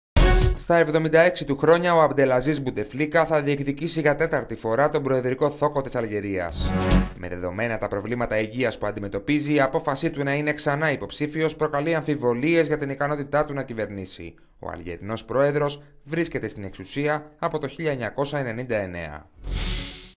Αλγερία: Τέταρτη θητεία θα διεκδικήσει ο πρόεδρος Μπουτεφλίκα
Στα 76 του χρόνια, ο Αμπντελαζίζ Μπουτεφλίκα θα διεκδικήσει για τέταρτη φορά τον προεδρικό θώκο της Αλγερίας.

Με δεδομένα τα προβλήματα υγείας που αντιμετωπίζει, η απόφαση του να είναι ξανά υποψήφιος προκαλεί αμφιβολίες για την ικανότητα του να κυβερνήσει.

Ο Αλγερινός πρόεδρος βρίσκεται στην εξουσία από το 1999.